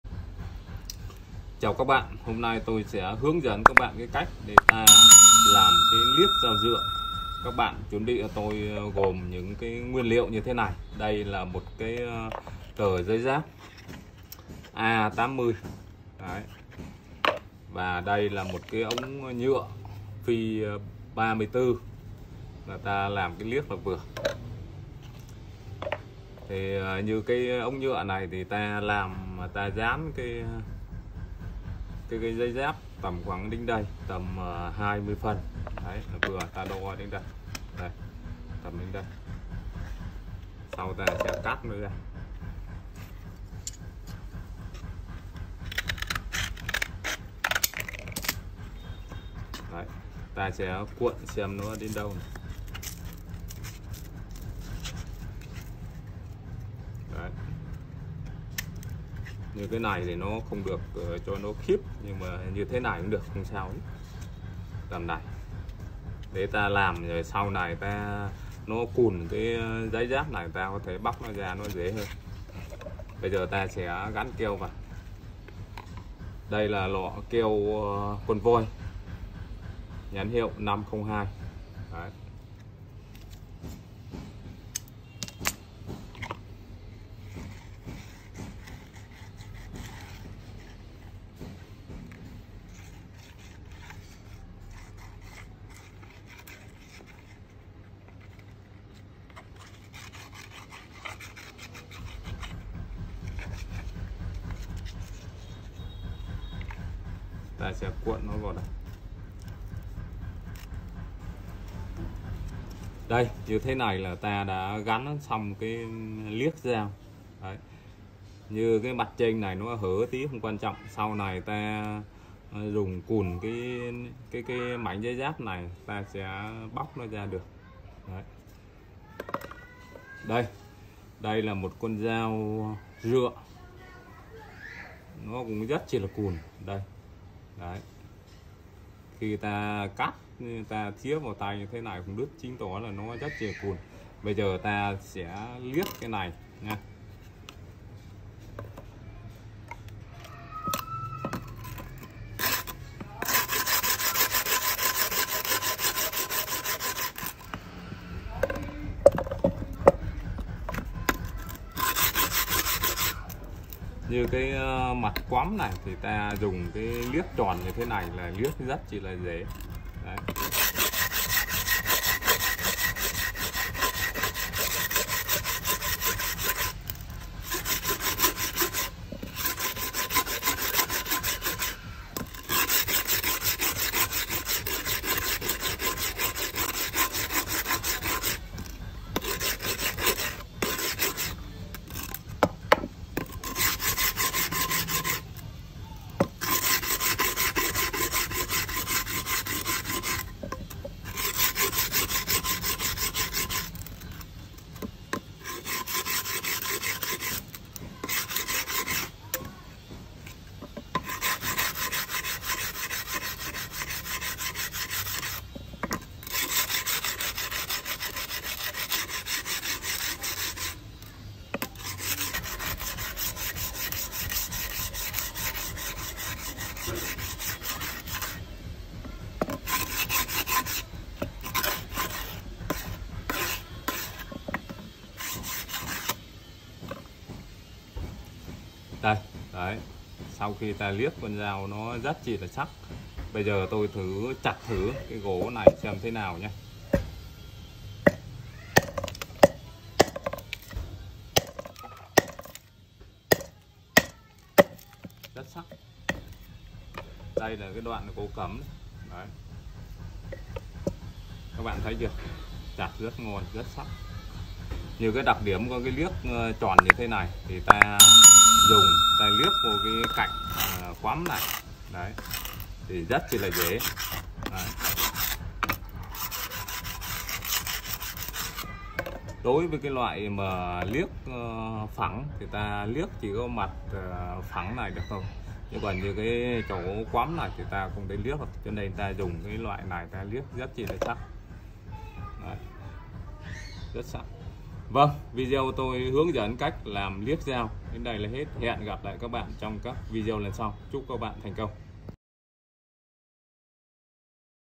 Cách làm liếc dao rựa sắc như dao cạo râu
phamgiangmaidao cachlamliecdaoruasacnhudaocaorau# tôi hướng dẫn cách làm liếc dao rựa này đơn giản không tốn kém mà rể mài rể liếc, nhưng cách này chỉ áp dụng cho những lúc ta đang chặt cây cùn mà không có đá mài ở bên cạnh hoặc ta không biết mài thì dùng cách này là hay nhất, nhưng có đá mài thì các bạn mài sắc hơn, nhưng so với cái liếc này thì liếc ở khoảng cong mũi dao thì rể hơn,chúc các bạn thành công… nếu có gì không hiểu thì commen xuống dưới video hoặc Zalo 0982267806 cho mình , mình xin được trả lời các bạn sớm nhất có thể…